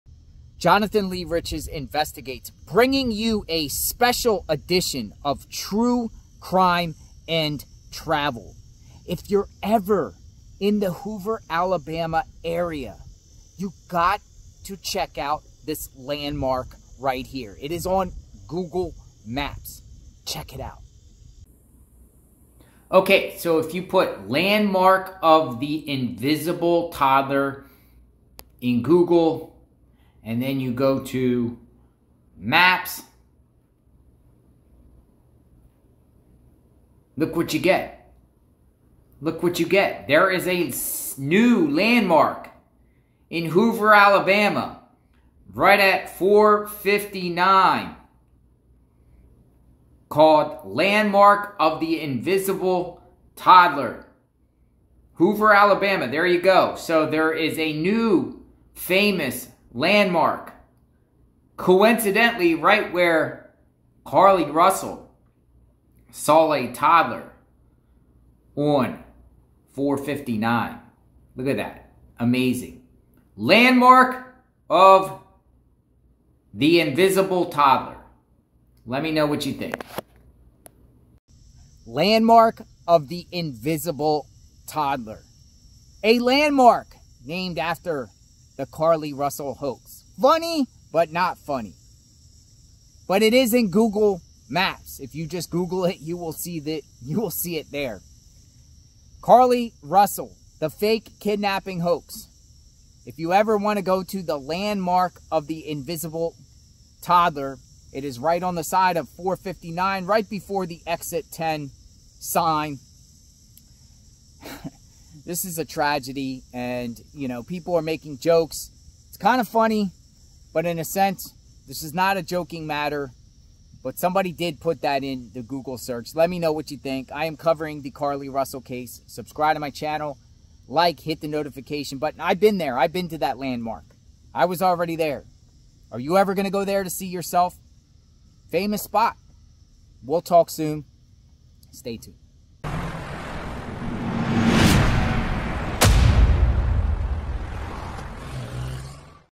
Carlee Russell & Landmark of the Invisible Toddler!
I am covering the Carlee Russell case.